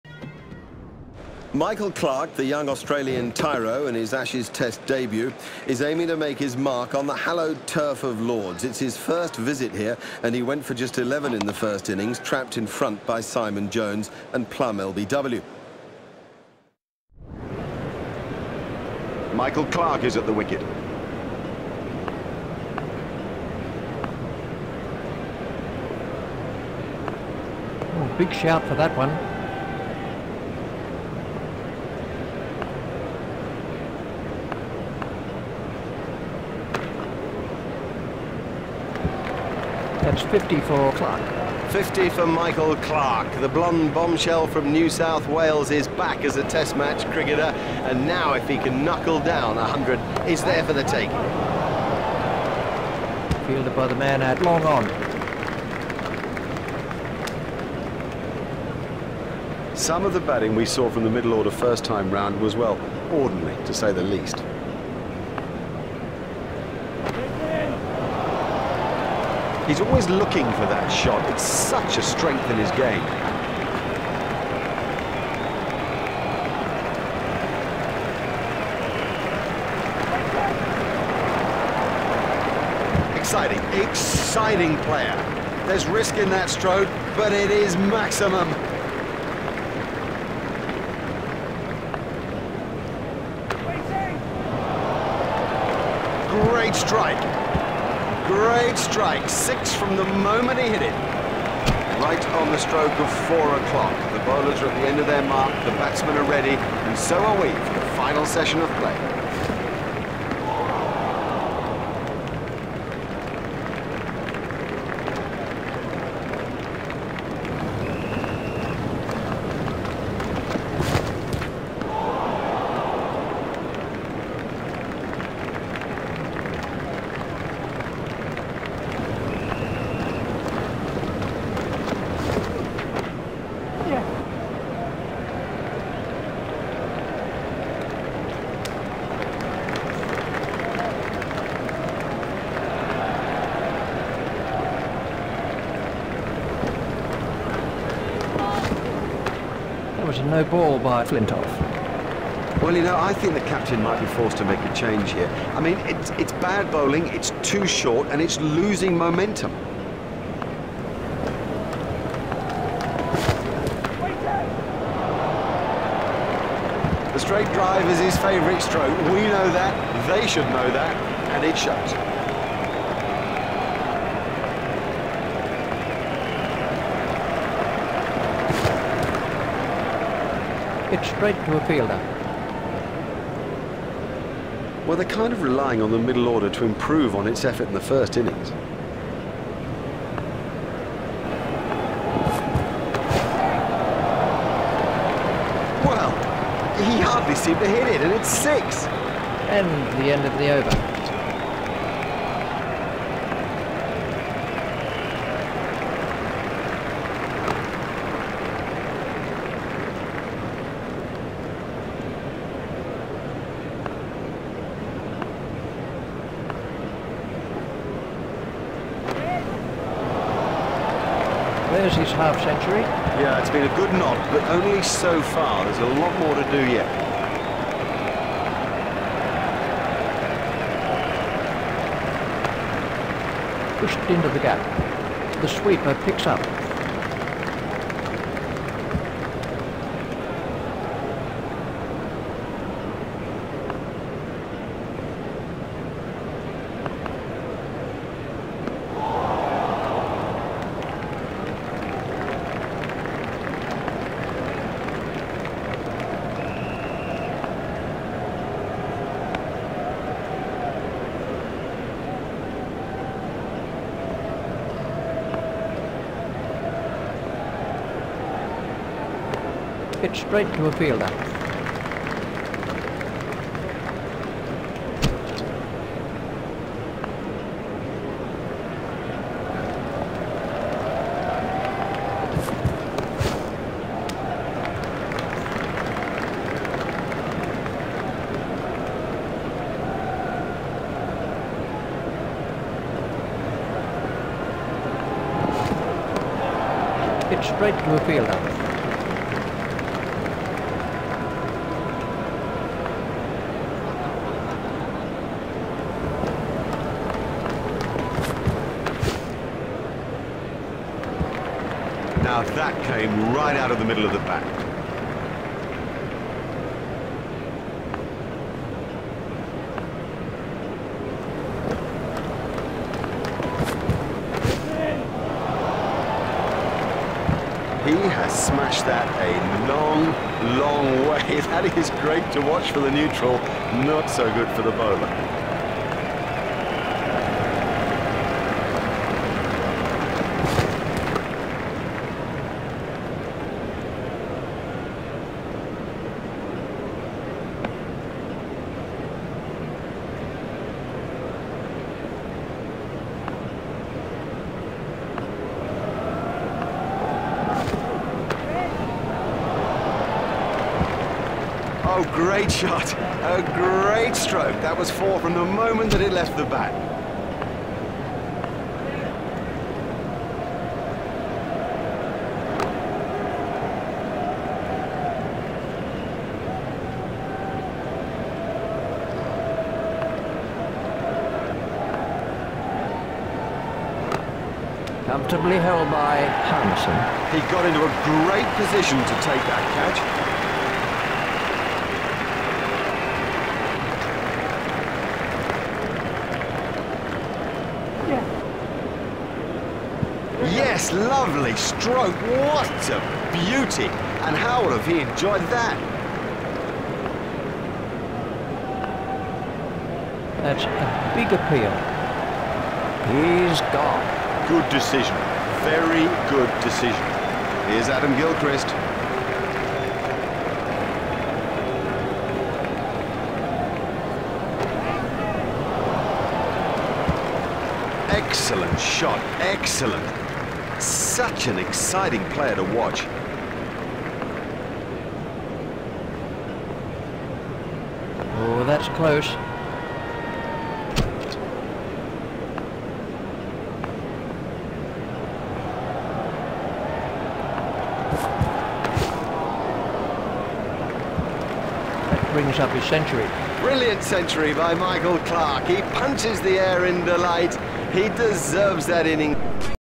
1st test at lord,s michael clarke has got off to a great start innings 100 runs
1st test at lord,s michael clarke has got off to a great start innings 100 runs